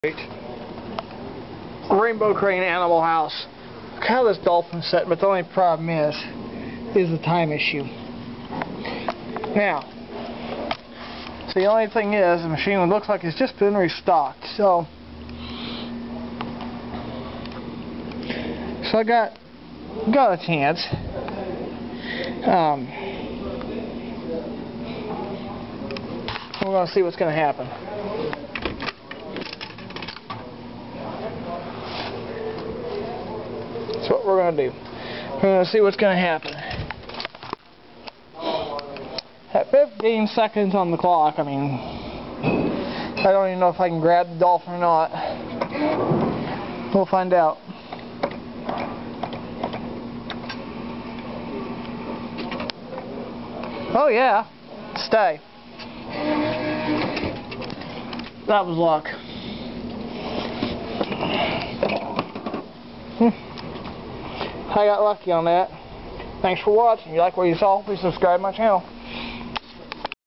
(RAINBOW CRANE) ANIMAL HOUSE CLAW MACHINE@SILVER DOLLAR CLAW LETS GO OF MY PRIZE OVER THE HOLE
TODAY I HEAD OVER TO MY OTHER LOCAL BOWLING ALLY TO FACE THE ANIMAL HOUSE CLAW MACHINE HERE IF U WATCH THE CLAW IT LOOKS LIKE THE CLAW OPENS UP AND LETS GO HOPE Y'ALL ENJOY